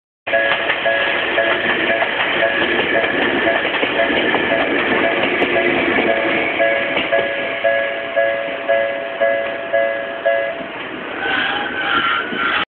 【HQ】東急電鉄池上線7000系 通過シーン 2
24日に秋葉原のじゃんぱらで買ったSoftBankの920SHで、何か動画撮影をしてみようという形で、蒲田駅前の踏切にて東急池上線と多摩川線の撮影をしました。最大で480pに対応してることから、画質はまあまあ良いものの、音質は微妙な感じです・・。ただ、画質重視であるなら、私が動画撮影でメインに使っているDMC-TZ7のバッテリー(またはメモリーカードの残量)が切れて使えなくなった時に、次なる手段として920SHでもいけそうですね。
ちなみに編成は7102Fです。